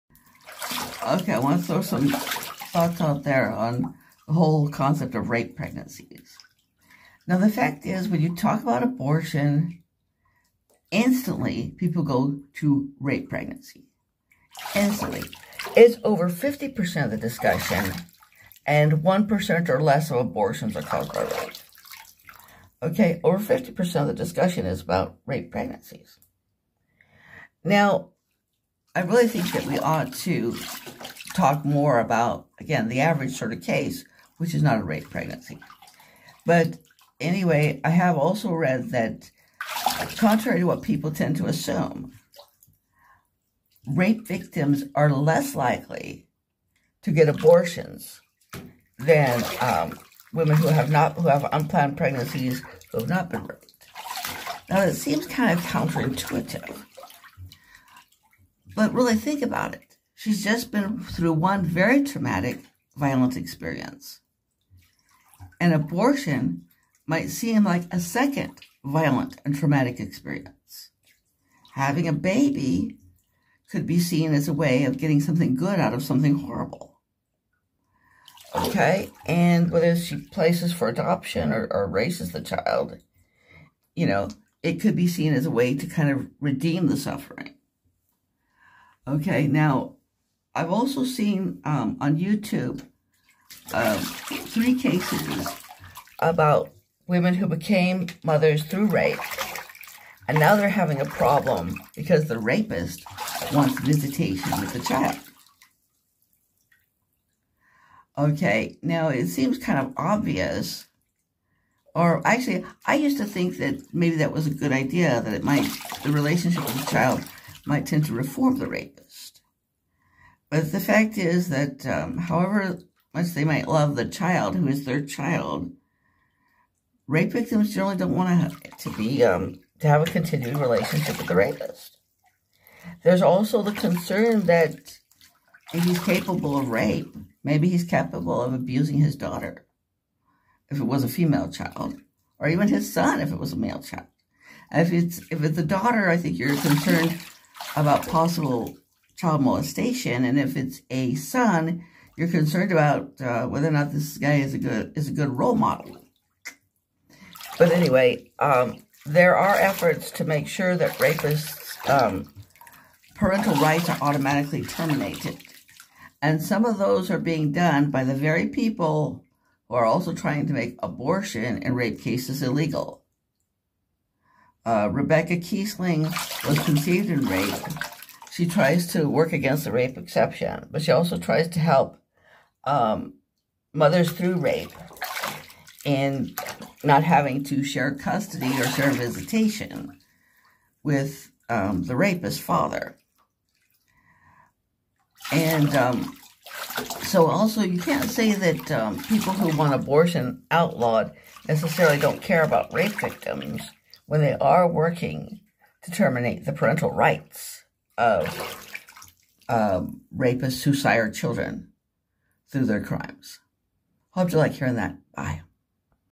Why Rape Victims Often Don't Abort
Rape is a very violent and traumatic experience. An abortion can be viewed as a very violent and traumatic experience -- explaining why rape victims are less likely than other girls and women with unplanned pregnancies to abort.